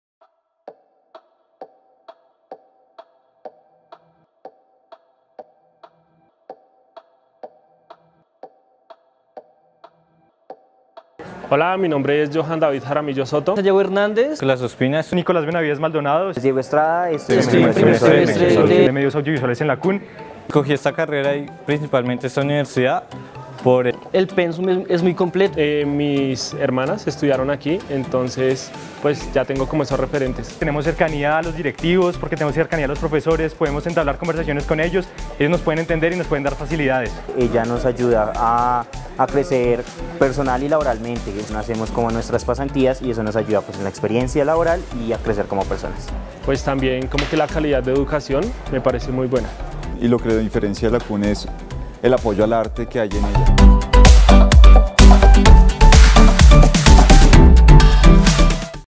Por qué estudiar Medios Audiovisuales en la CUN
Estudiantes del Programa de Dirección y Producción de Medios Audiovisuales
nos cuentan porque estudiar en la Corporación Unificada Nacional de la Educación Superior - CUN.
Sede Principal A - Calle 12B N° 4 – 79 l Bogotá, Colombia Sede F-  Medios Audiovisuales  - Calle 12C N° 3-99 (Antigua Calle 14 N° 3-99)